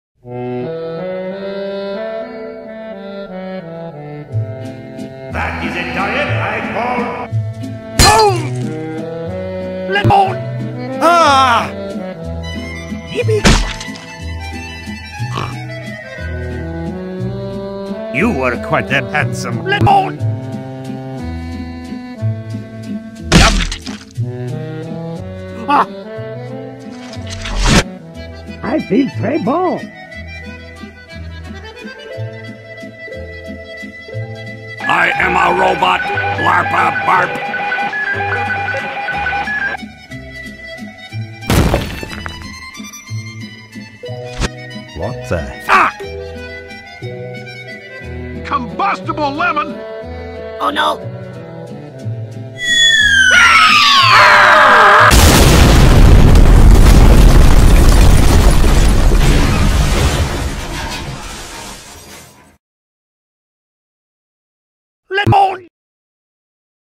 [Old] Spy Also Loves Lemons
An old animation created years after seeing Spy Loves Melons by none other than @theTig3r42 .

Props to you, man.

You need to enable "Show mature content" in your app's settings to find NSFW videos.